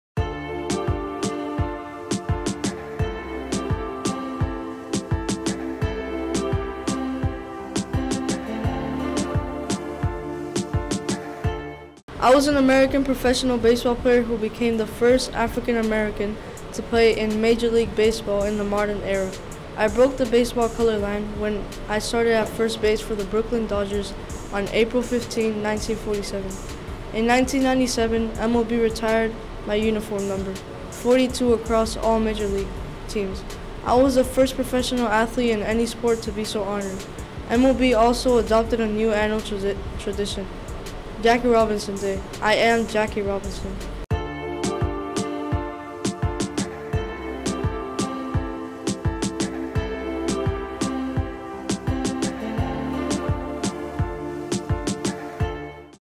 Black History Month Jackie Robinson
First base for the Brooklyn Dodgers, he broke the baseball color line in 1947.